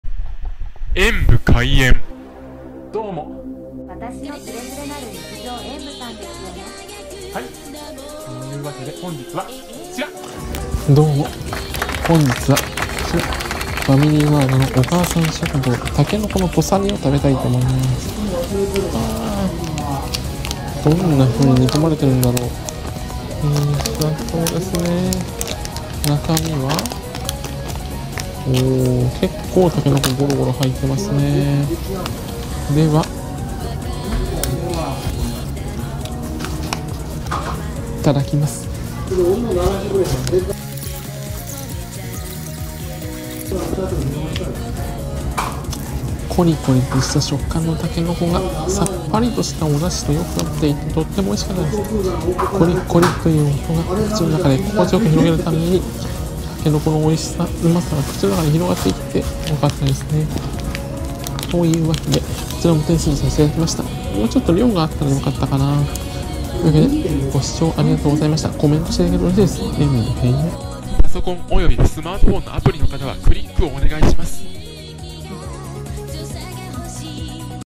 ファミリーマート お母さん食堂筍の土佐煮を食べてみた。これぞ和の煮物！！
ファミリーマート　お母さん食堂筍の土佐煮を食べてみました。
これぞ和食の煮物！！という味わいですね～ボリボリが美味しい。
贈り物は此方まで↓
〒153-0042